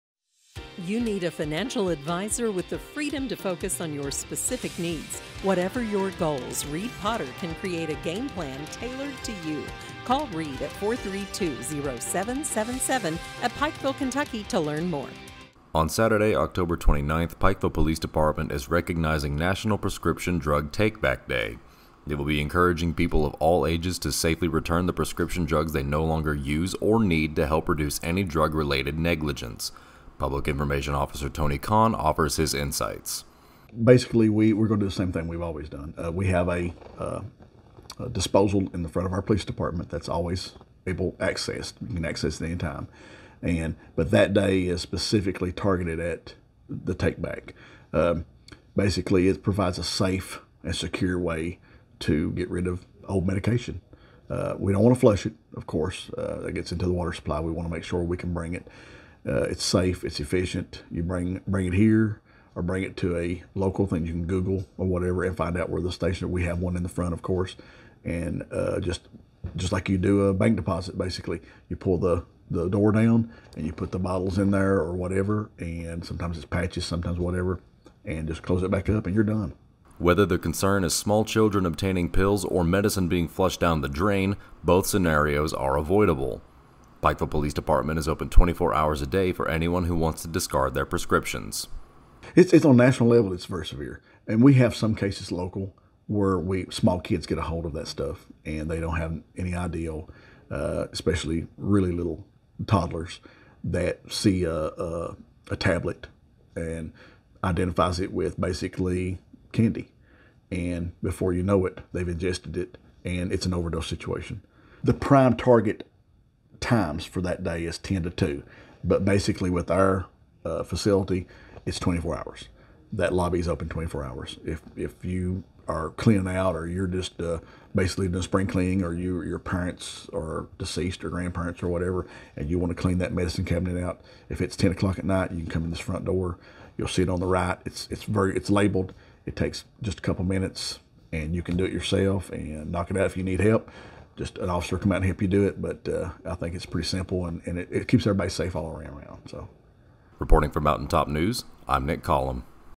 National Prescription Drug Take Back Day is October 29
The Pikeville Police Department is recognizing National Prescription Drug Take Back Day on Saturday, October 29, and is encouraging people to safely return unused and unneeded prescription drugs.